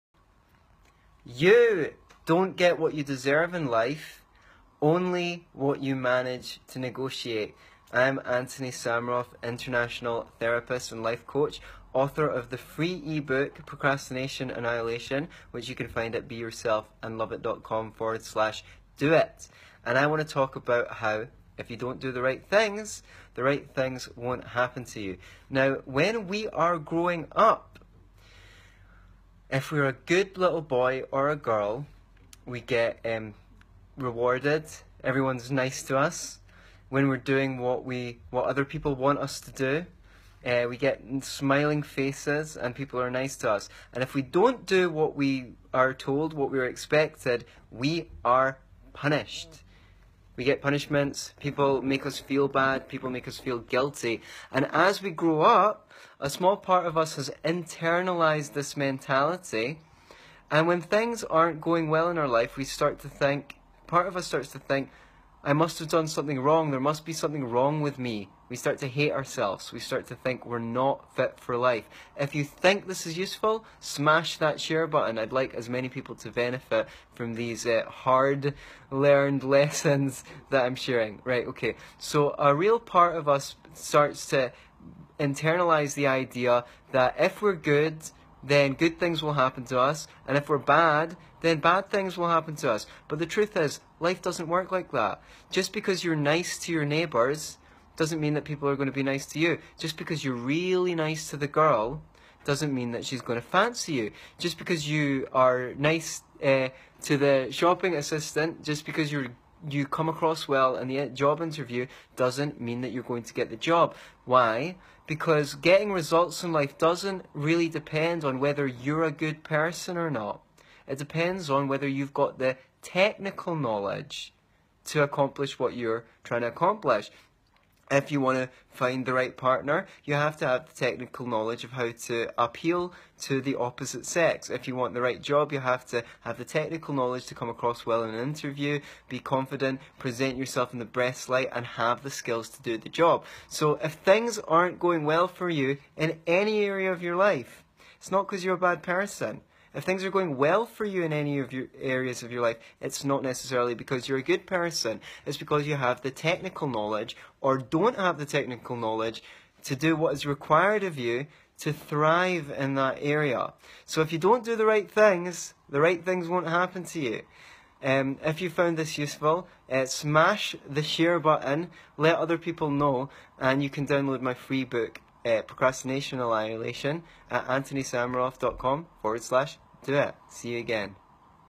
If you don't do the right things the right things won't happen to you!
Antony Sammeroff explains why you don't get what you deserve in life only what you manage to negotiate.

Join my personal development course and change your life

Download my Ultimate Communication Power! CD for free to join my mailing and get free updates: